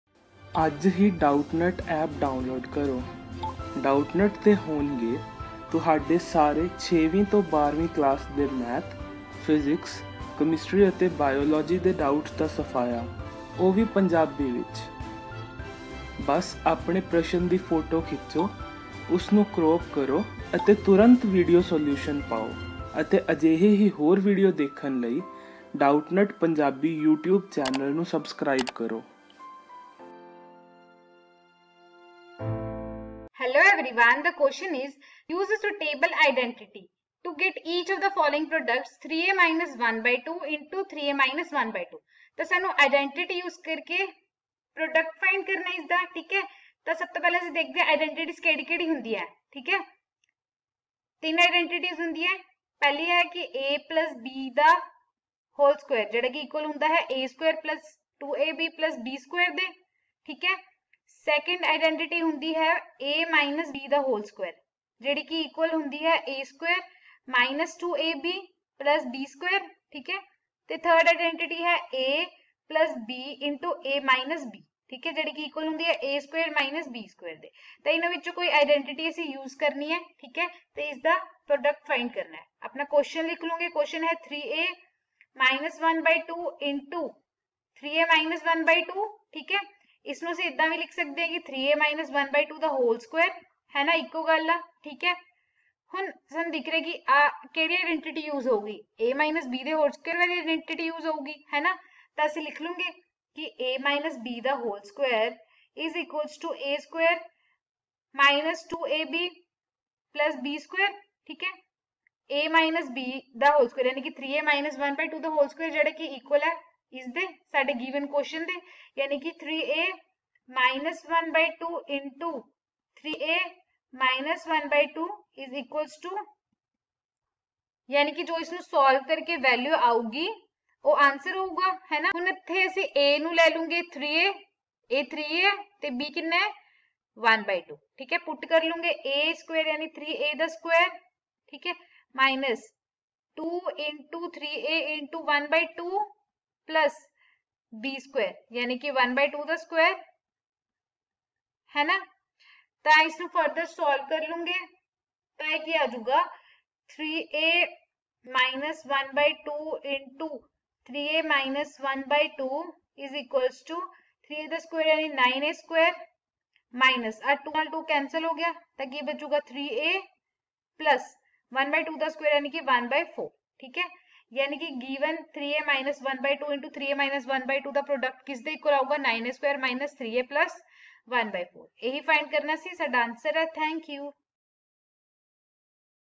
Use a suitable identity to get each of the following products : `(3a-1/2)(3a |Class 9 Math |Doubtnut
Use a suitable identity to get each of the following products : `(3a-1/2)(3a-1/2)`

Online Class में Admission लेने के लिए यहाँ click करें :
👉Class 8 | English Medium | Tuition Classes :

👉 Have Any Query? Ask Us.